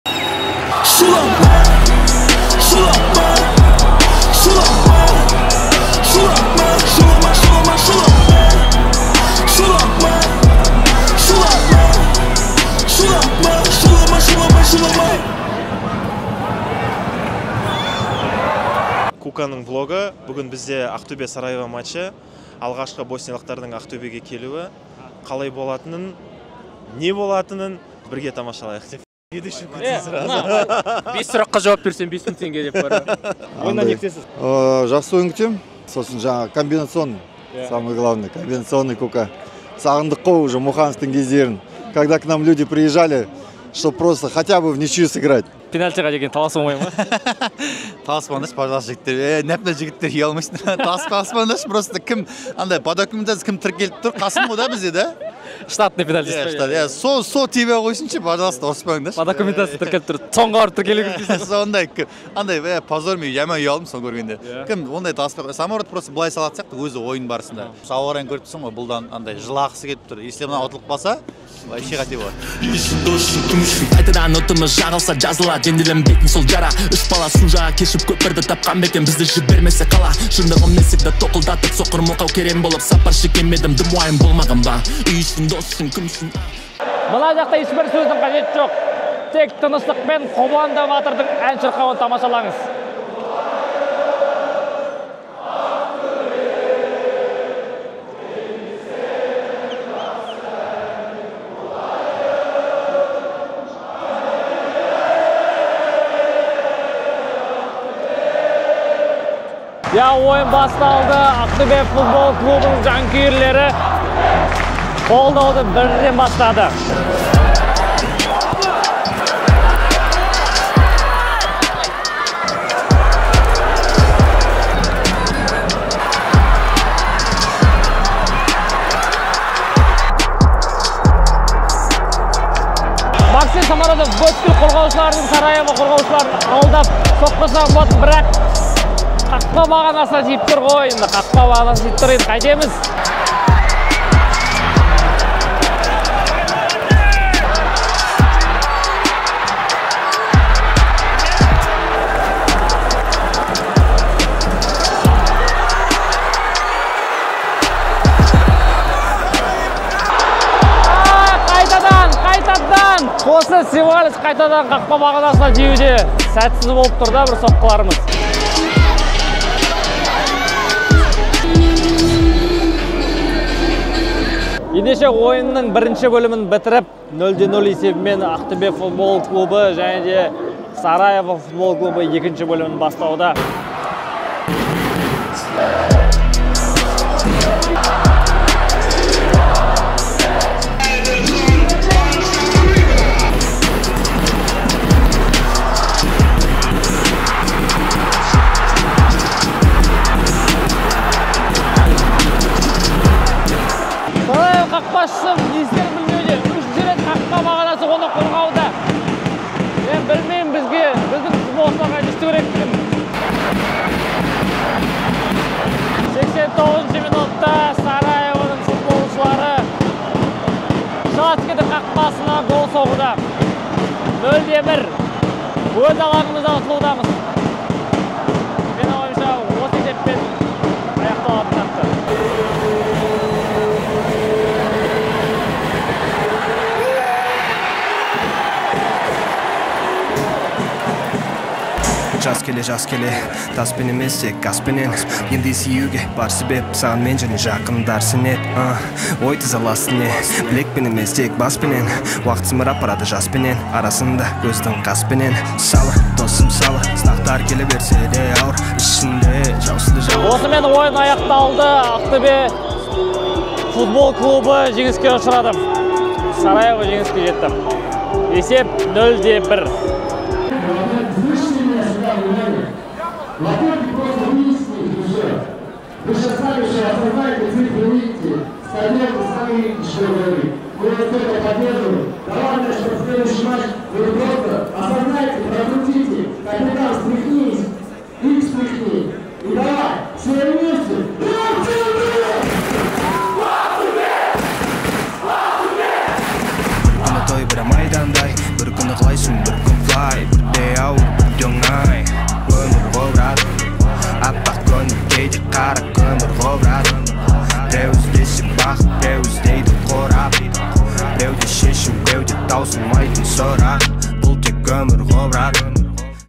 Қазақстанда болмаған перфоманс | Ақтөбе - Сараево
Ақтөбе - Сараево ойынынан шолу.
Ақтөбе өз алаңында ұтылды.
Қазақстанда болмаған перфоманс.